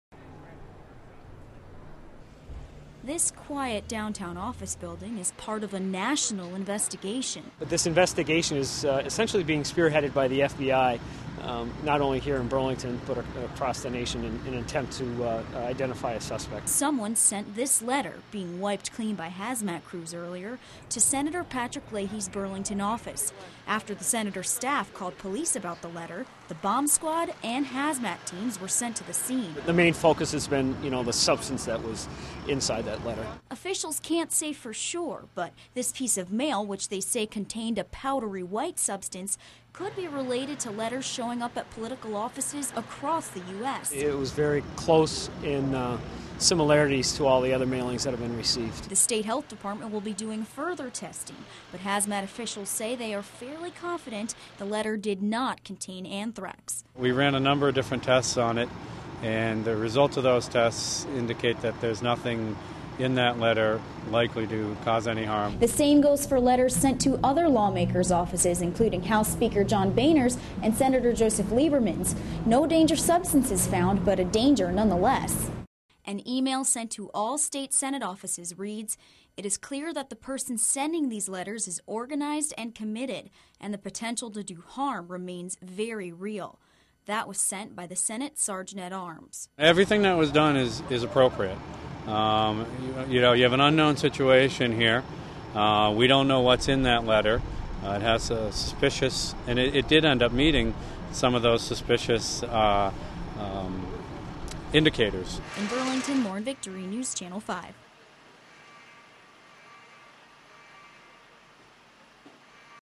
Suspicious Package Tested, Cleared At Leahy's Office
US Capitol Police and the Burlington Police Department investigated the receipt of a suspicious package at Senator's Leahy's office in Burlington Thursday.